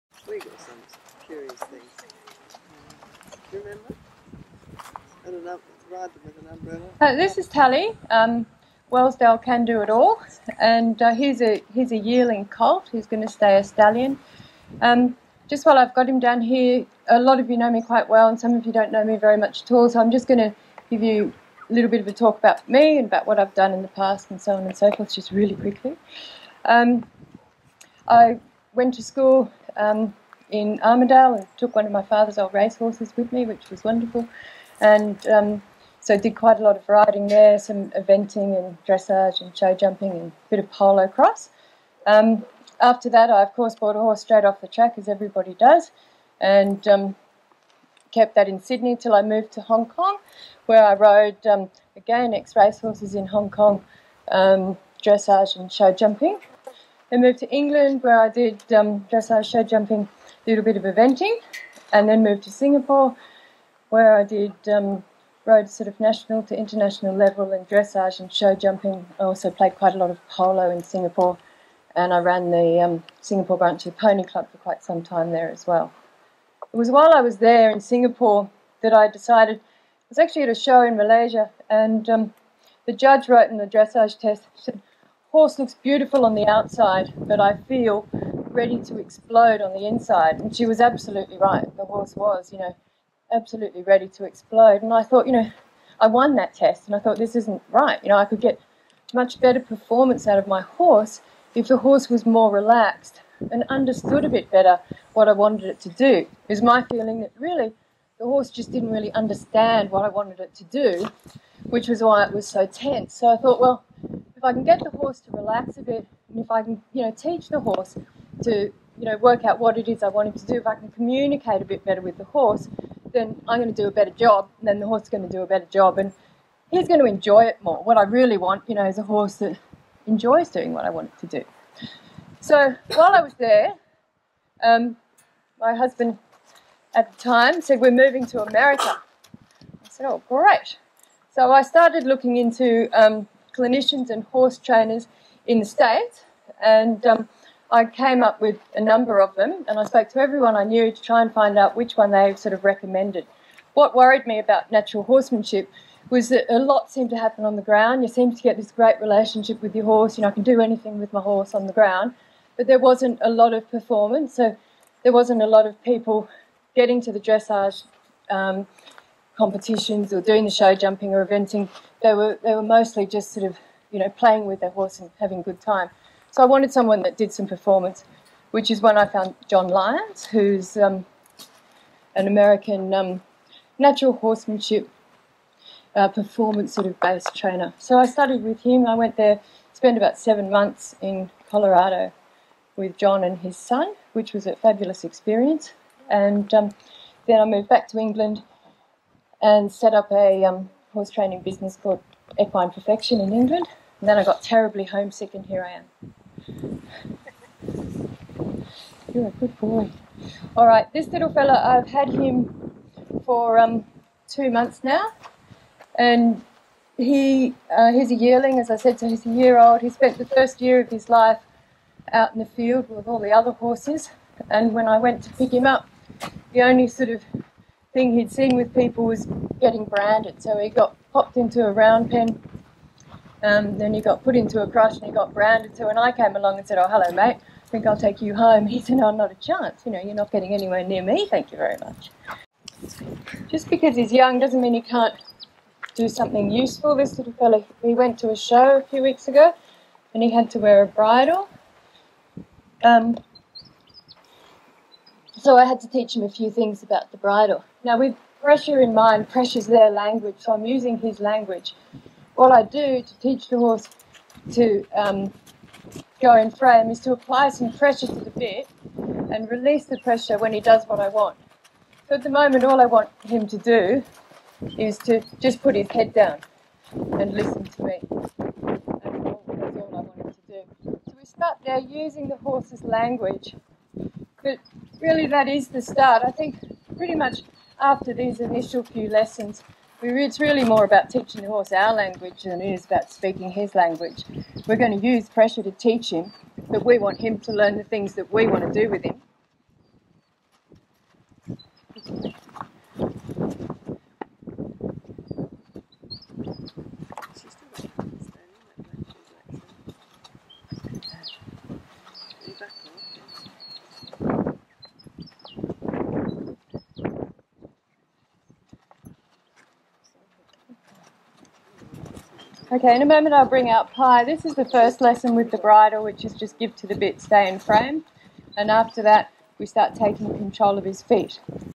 Young horse at demo
It's interesting that the basics of the Kandoo training haven't in nine years. Although I have refined the lessons and found easier ways to teach both horses and riders/handlers, the foundations remain the same.
I came across this old demo video when I was selling this ten-year-old stock horse gelding earlier this week. It was wonderful to see him here at this demo as a yearling, learning quietly and calmly, which is how all of his training progressed.
As a result, as a ten-year-old horse, he is quiet, relaxed, accepting and eager to learn new things - the perfect equine partner.